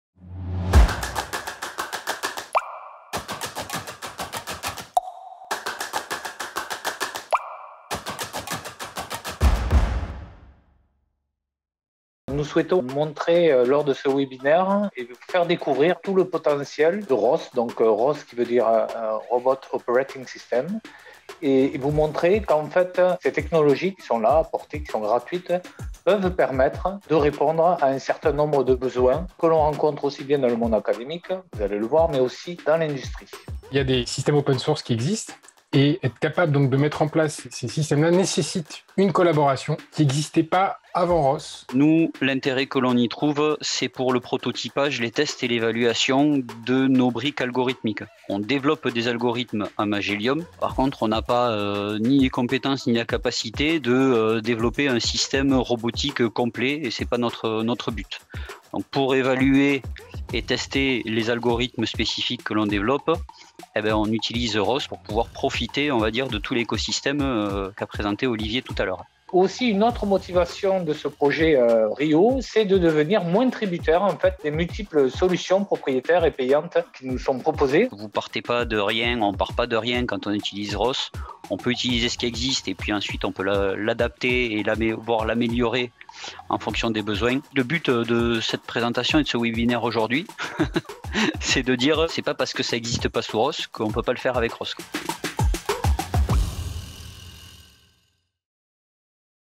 Webinar S.Mart | L’intégration robotique sous ROS dans l'Industrie du Futur (résumé)
La robotisation de certaines tâches de production est aujourd'hui davantage freinée par les coûts liés à l’intégration robotique que par ceux liés aux investissements matériels.
Chaque système robotique possède en effet ses spécificités et son environnement de développement propre.
De ce fait, intégrer plusieurs matériels pour la réalisation d’une tâche requiert de l’expertise dans chacun des environnements de développement et des investissements d’ingénierie conséquents pour les mettre en relation.
Le middleware gratuit ROS (Robot Operating System) permet de libérer l’ingénieur des contraintes liées aux environnements propriétaires payants et de disposer d'un environnement unique pour intégrer rapidement des systèmes hétérogènes.
ROS capitalise des années de développement de robotique et les met à la portée de tous les utilisateurs.

Ce webinaire dresse un panorama de ROS et propose des retours d’expériences industriels et académiques sur son usage (potentiel et maturité, cas d'usage possibles au sein des pôles S.mart ou ailleurs...)..